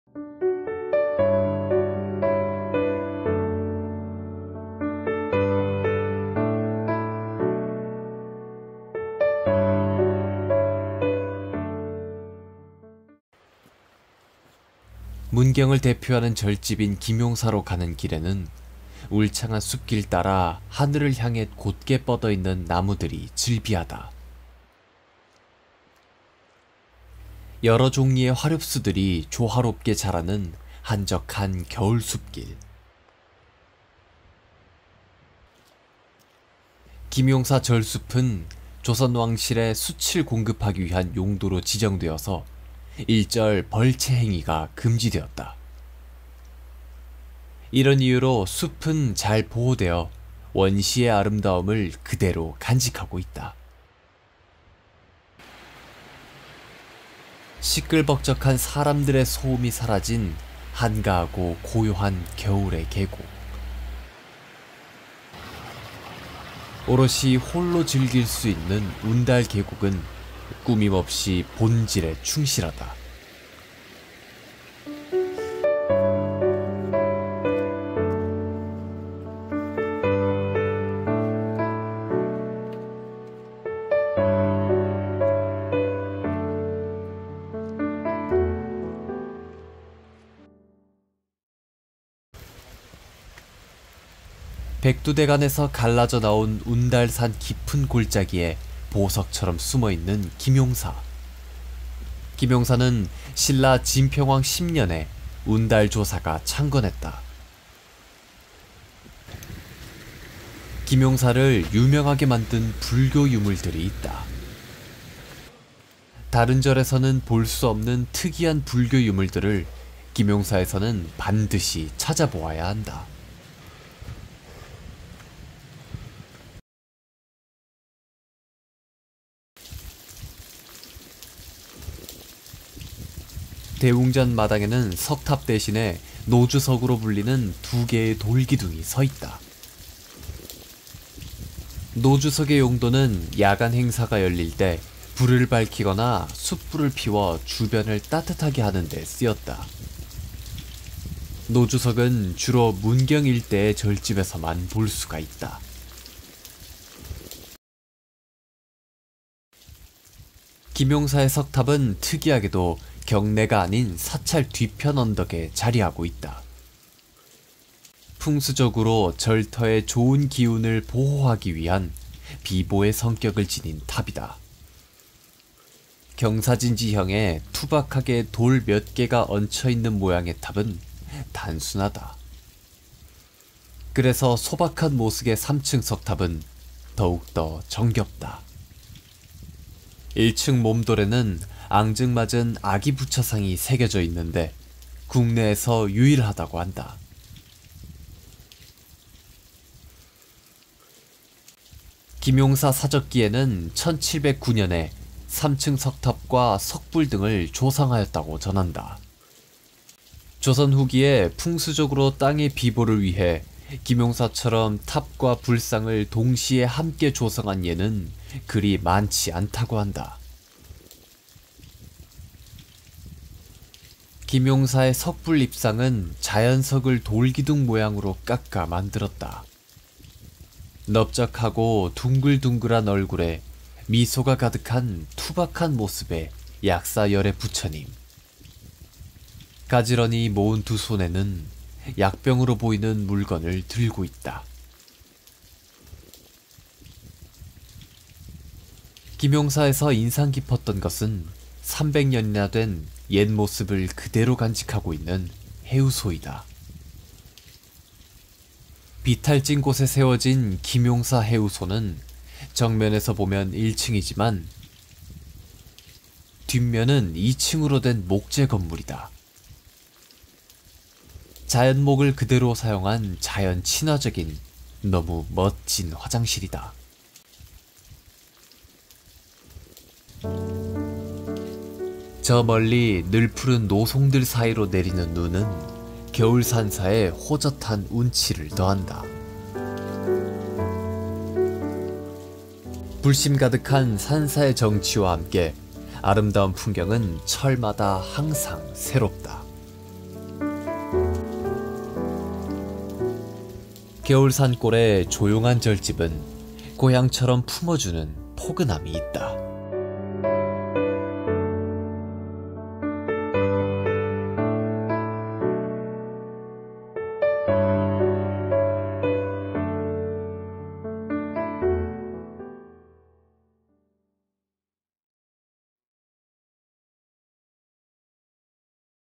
【문경 김용사에서 만나는 특이한 불교 유물들】 김용사「천년숲길」 「운달계곡」 / 산사여행136번째
백두대간에서 갈라져 나온 운달산 깊은 골짜기에 보석처럼 숨어 있는 문경 김용사.
김용사는 신라 진평왕 10년에 운달조사가 창건 했다.(더보기를 눌러주세요)
김용사를 유명하게 만든 불교 유물들이 있다.
다른 절에서는 볼 수 없는 특이한 불교 유물들을 김용사에서는 반드시 찾아 보아야 한다.

 (재)전주정보문화산업진흥원/공유마당

*기획•촬영•편집 : 도재TV
  (영상이 필요하신분은 연락주세요.)